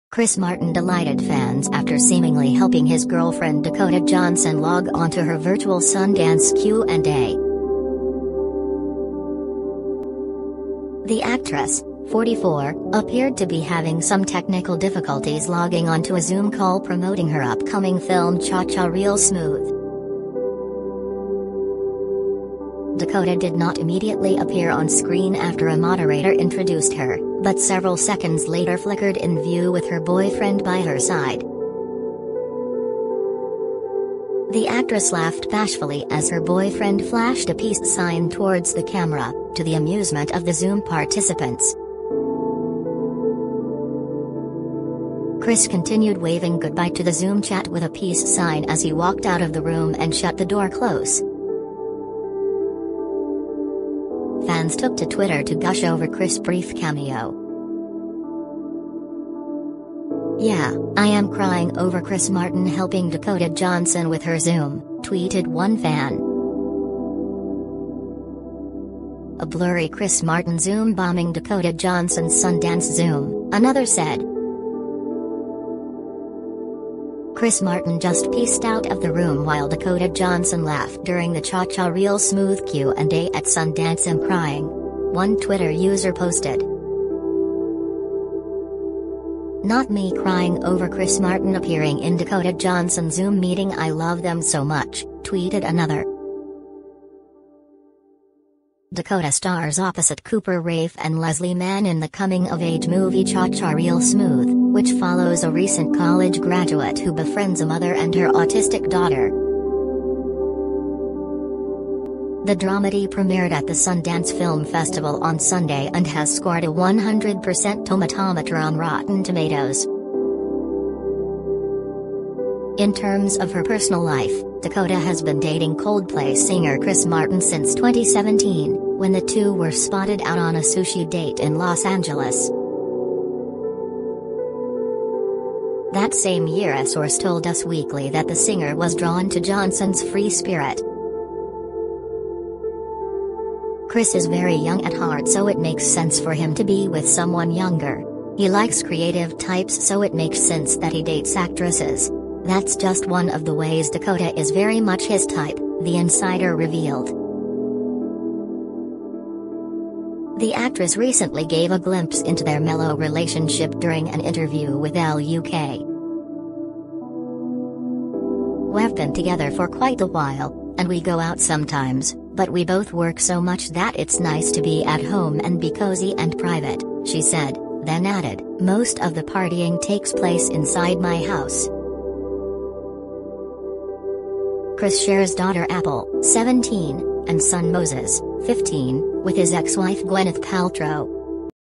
Chris Martin delights fans after seemingly helping girlfriend Dakota Johnson log onto her Sundance Z
Chris Martin delighted fans after seemingly helping his girlfriend Dakota Johnson log onto her virtual Sundance Q&A.
The actress, 44, appeared to be having some technical difficulties logging onto a Zoom call promoting her upcoming film Cha Cha Real Smooth.
Dakota did not immediately appear on screen after a moderator introduced her, but several seconds later flickered in view with her boyfriend by her side.
The actress laughed bashfully as her boyfriend flashed a peace sign towards the ca